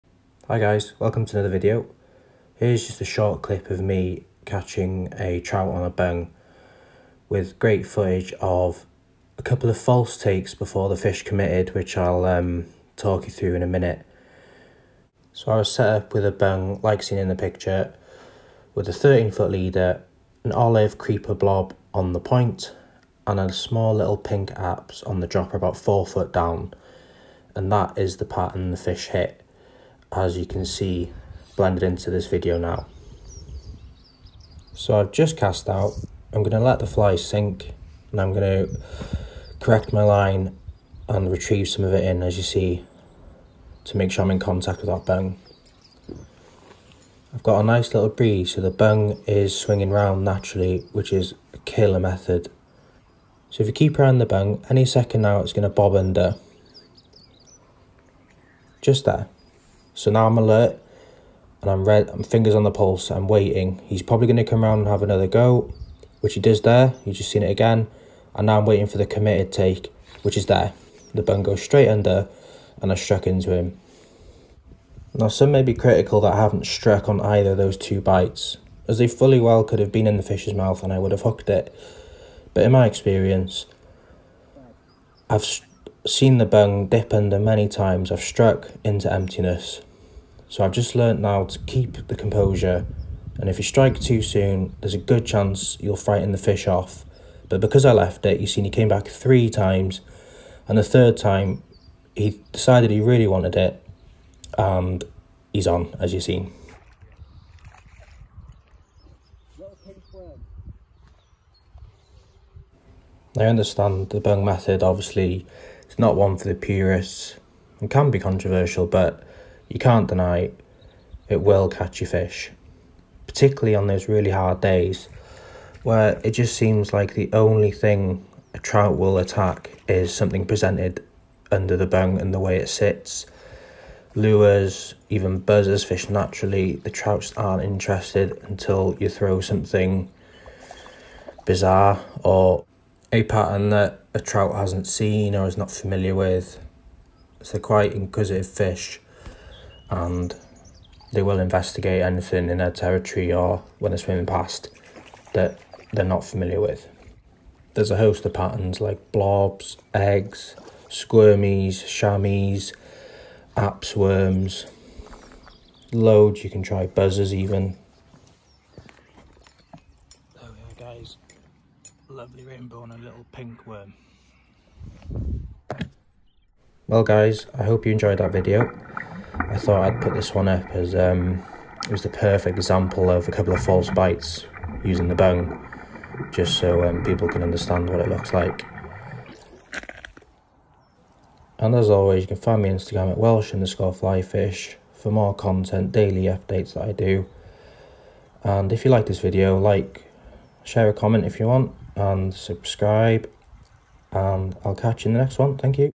Fishing the bung method and detecting ‘false takes’ fly fishing for trout.
Hi guys,

I hope you enjoy this small video of catching a trout using the bung.

Here I am fishing on a Saturday morning at my local graiglwyd springs.

It’s a perfect example of witnessing a trout ‘pecking’ or sipping in and out a fly that’s making your bung dip under for a split second, if you can hold your nerve and be patient, more often than not the fish will return if it likes what it sees and fully commit to taking the fly as you can see in this clip.

Controversial method no doubt however it’s a very efficient way of catching fish and I would recommend introducing this method to beginners as we want them to be straight into the action to fall in love this the sport that we all do.

And there are instances where the trout just aren’t playing ball to any method until you show them something under a bung.

At the end of the day we’re all out to catch fish, and it’s better to catch a fish on the bung method than blank in my opinion,  I know some people will still disagree but that’s fine, each to their own.

If I was to be completely  honest, I only sought to use the bung if I’m a couple hours in and had no action or a sniff, it’s never my go to method but you can look at it as a ‘get out of jail’ solution at times.

Jake.